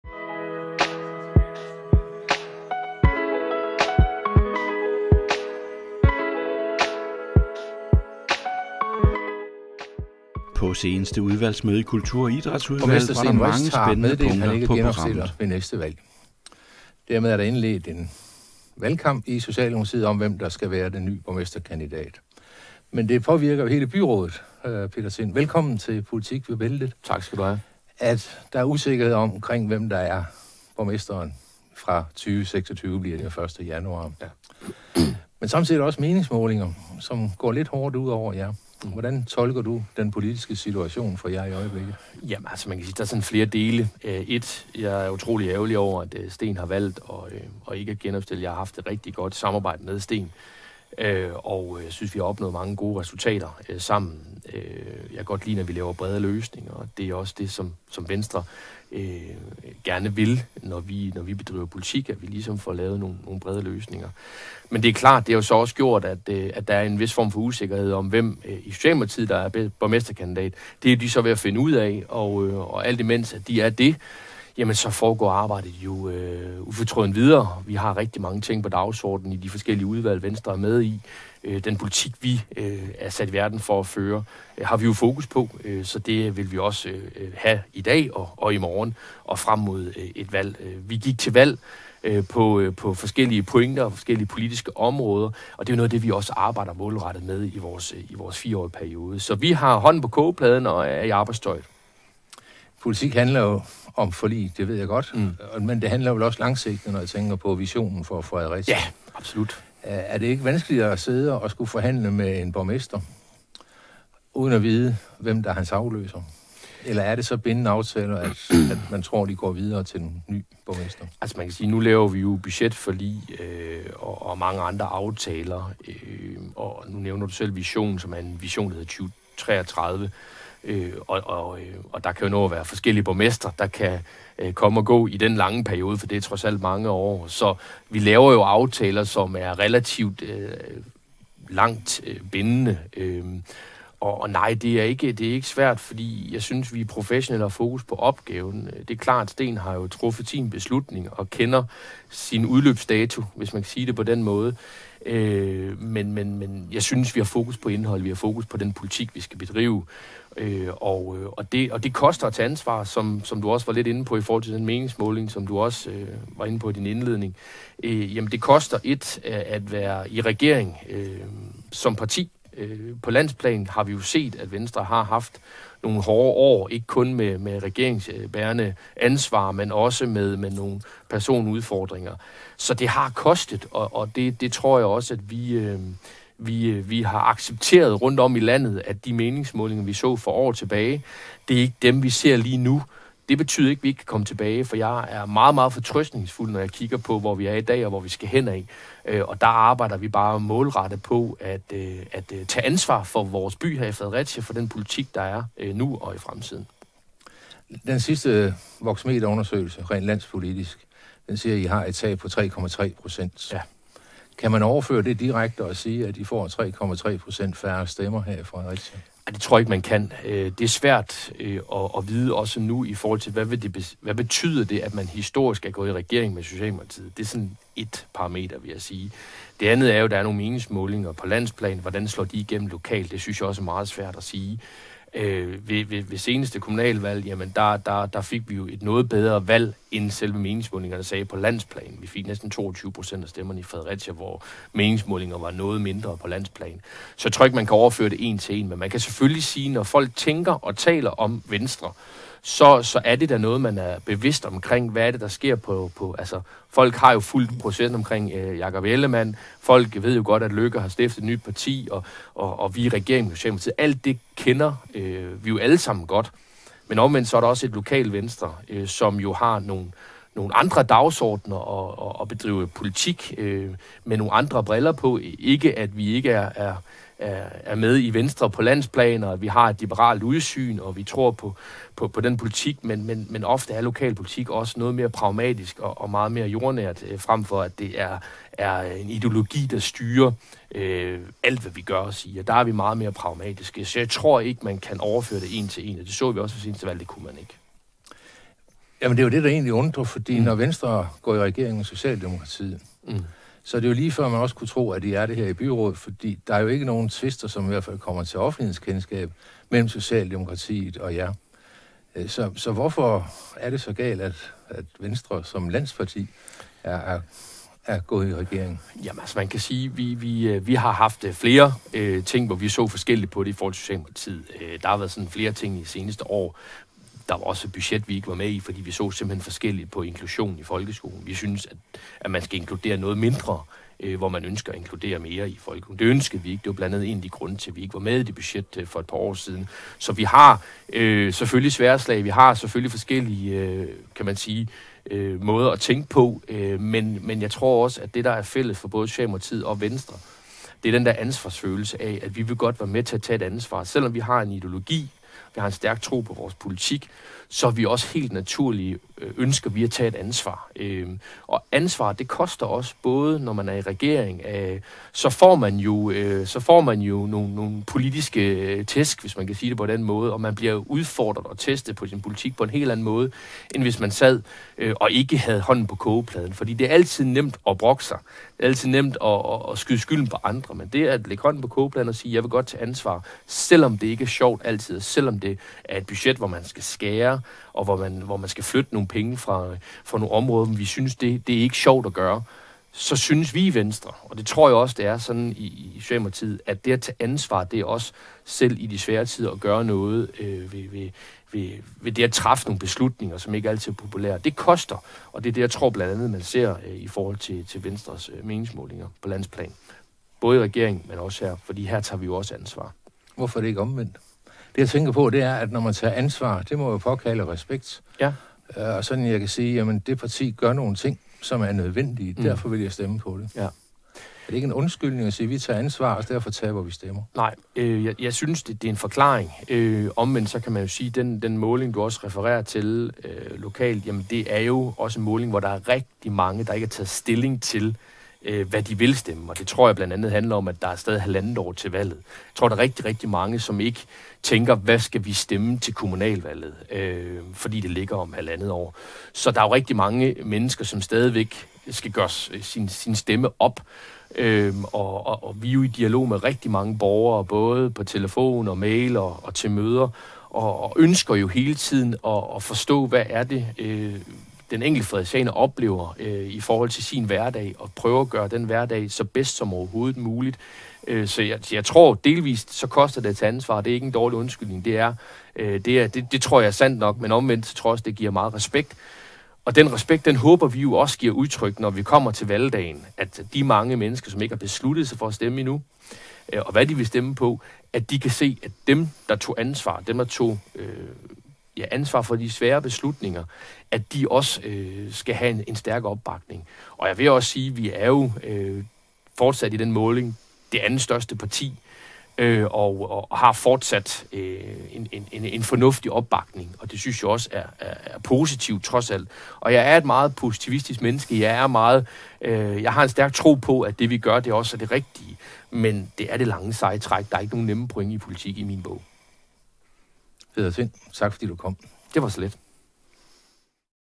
Voxmeter er den ugentlige meningsmåling. Peder Tind kommenterer
Mange politikere afviser at gå op i meningsmålinger - kan det nu også passe?
Peder Tind, byrådsmedlem i Fredericia for Venstre nægter i hvert fald ikke, og fortæller i denne udsendelse om meningsmålinger, skolevalg og det endelige valg, som er det helt afgørende.
DanmarkC TV bruger Voxmeter, og deres målinger sker i samarbejde med Ritzau.